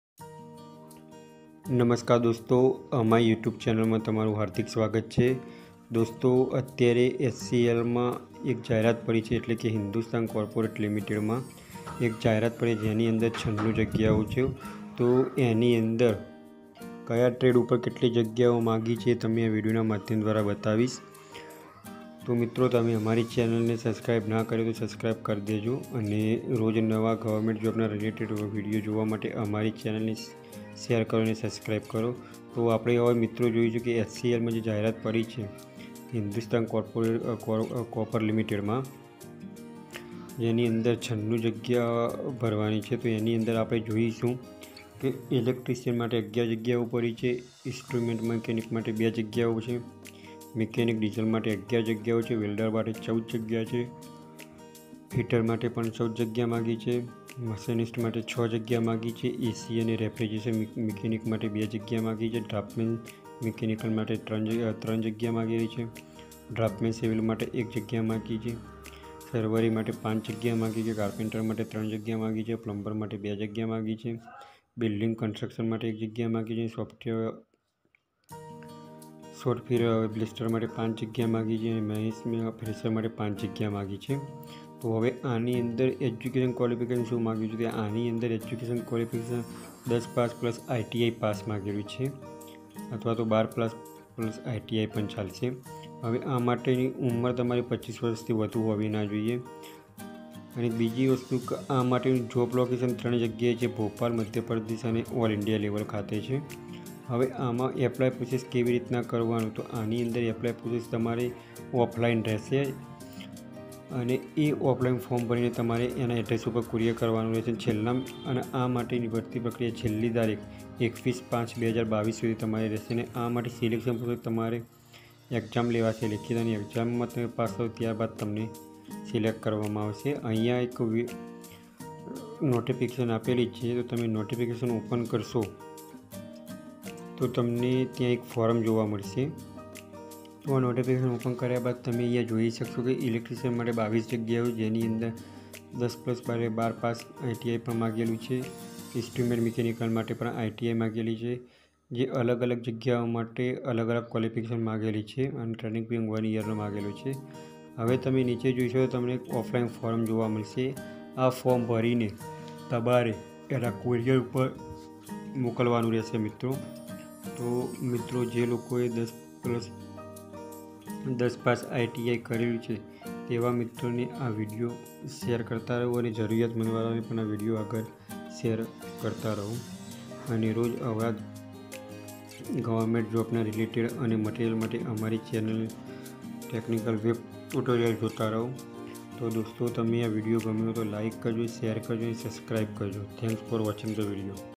HCL Recruitment 2022: Apply Online for 96 Trade Apprentice Posts
Hindustan Copper Limited (HCL) has invited online applications for the 96 Trade Apprentice post  for undergoing Apprenticeship Training at Hindustan Copper Limited, Malanjkhand Copper Project under the Apprentices Act 1961. Interested and eligible candidates can apply for these posts on or before  21 May 2022.